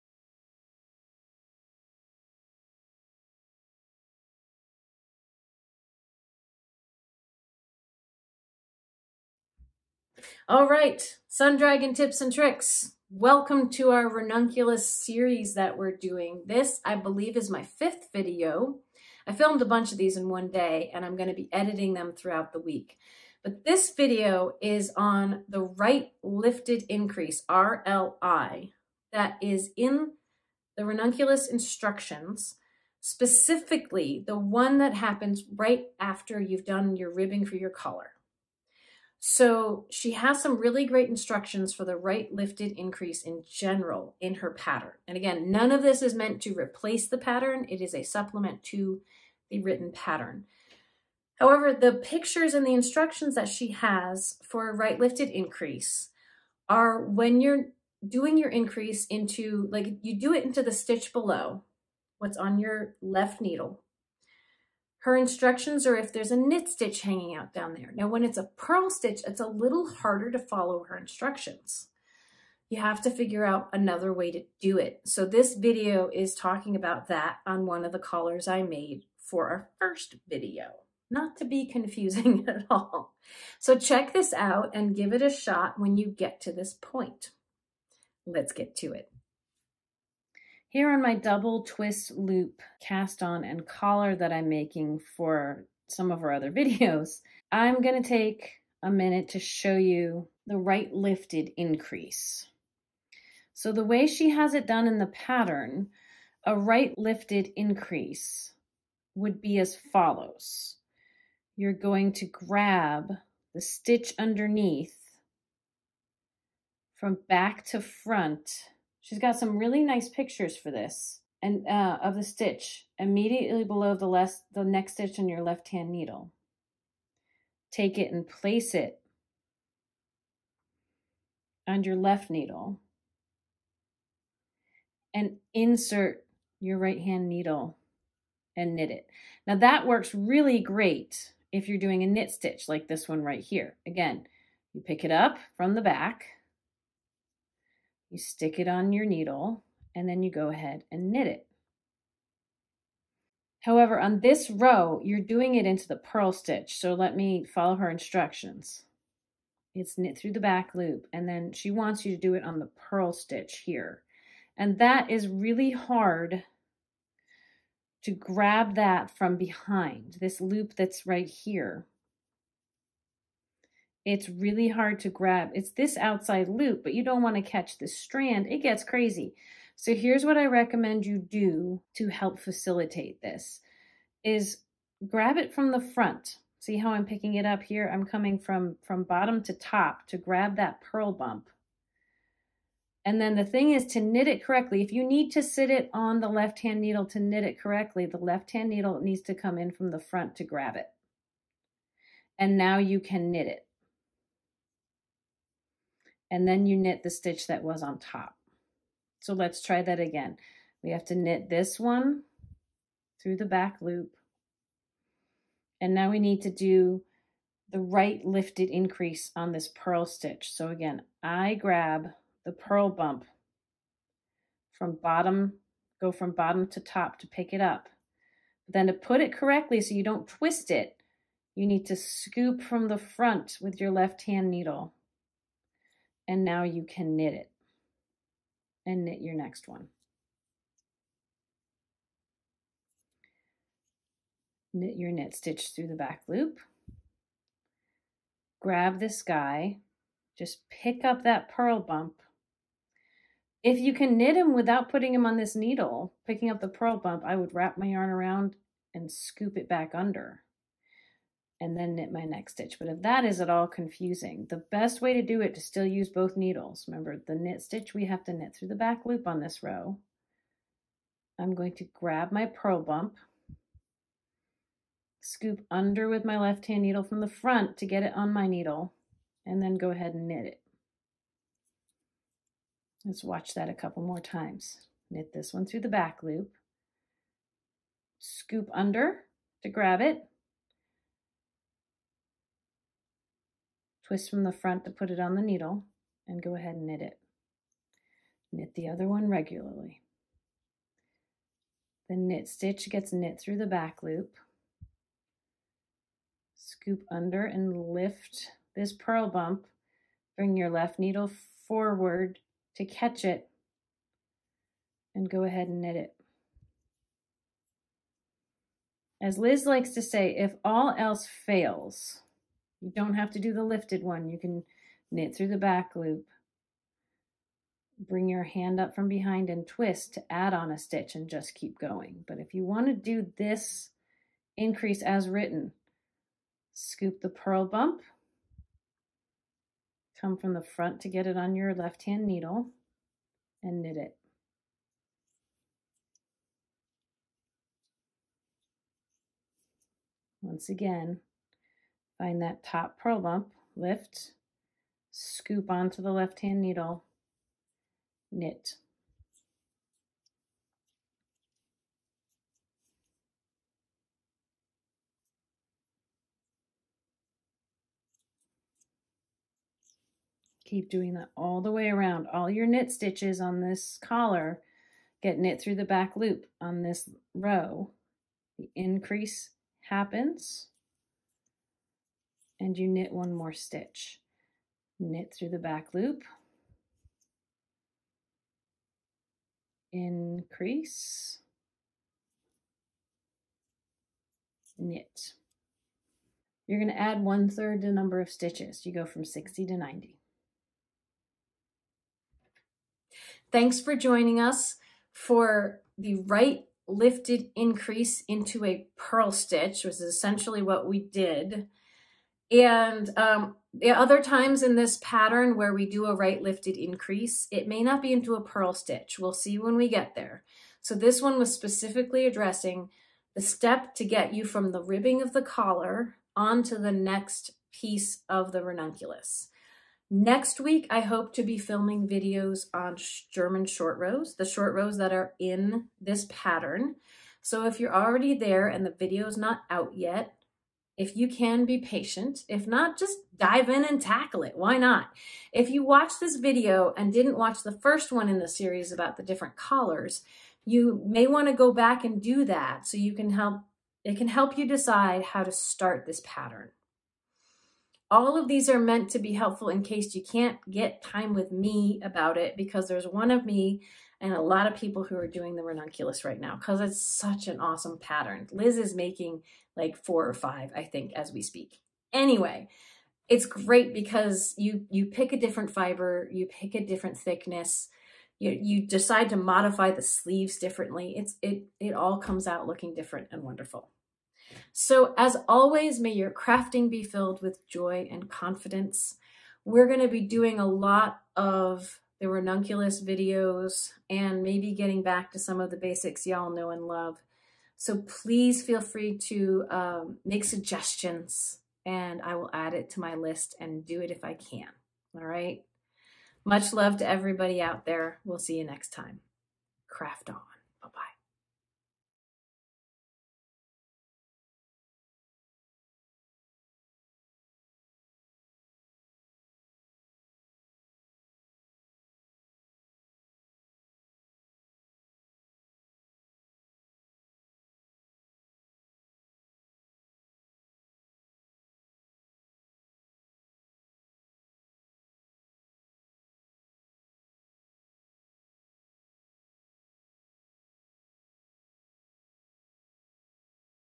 Knitting: Ranunculus Knit-a-Long help, part 5: RLI into the purl stitch of a ribbing
Sun Dragon is starting a knit-a-long in April 2022 for the Ranunculus sweater by Midori Hirose, and I'll be attempting to keep up with some videos that can assist the pattern. I will try not to reveal too much of the pattern, you can get your own copy from the pattern writer on Ravelry (about $6.50):

We love this pattern in the shop because it is so customizable, you can make it on any yarn from a lace weight to worsted weight, can alter the neckline, sleeve length, and torso length, and since it's designed to have enormous positive ease, you can do all of it on a size 10 needle, often without testing your gauge.

This video is the fifth in the series, laying out as clearly as I can the Right Lifted Increase specifically when done into a purl stitch, which may be challenging to do when following her instructions, set up for a knit stitch.

I'll be doing my best to build a playlist of videos that I've already make that could be helpful as well.

We will be talking a lot about the Ranunculus as well during this knit-a-long over there:

Let me know what else you would be interested in seeing in a video :)